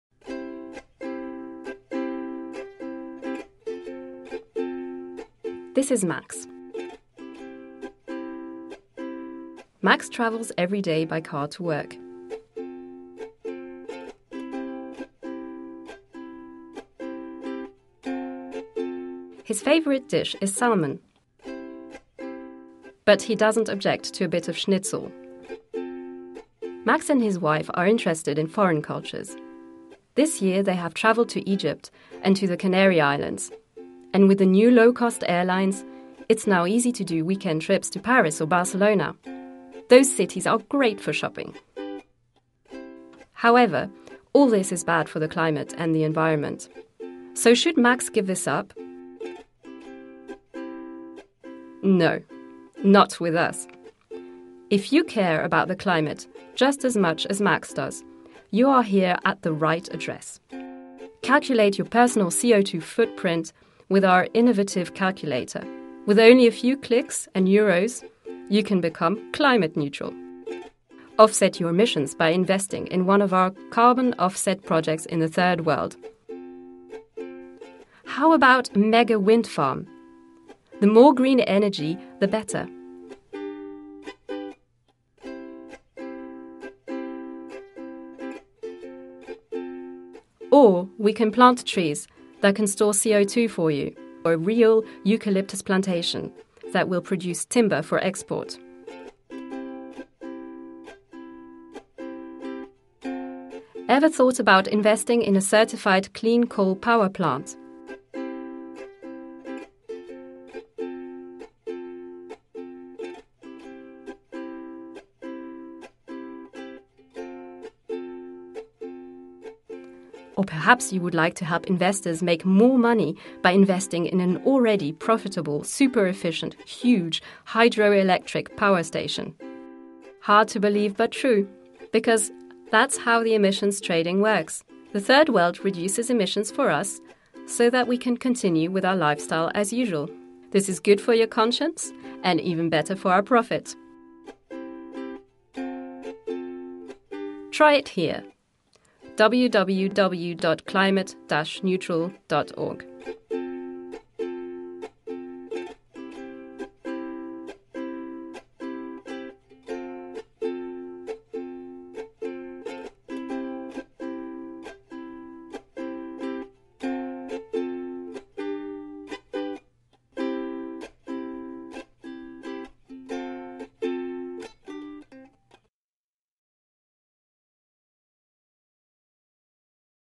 climate neutral right now - help prevent the climate change!
Climate neutral now!
Help prevent climate change!
Climate Neutral has the solution: With only a few clicks, you can redeem yourself and become CO2-neutral!
So you can continue to produce as many emissions as you want, by flying and driving. And by the way you prevent climate change.
This is good for your conscience and even better for our profit.
It seems too good to be true? Try it out.

...You want to help mitigate climate change? Then do something useful – without “redeeming” yourself. For example support the petition “No aviation growth! No false climate solutions!”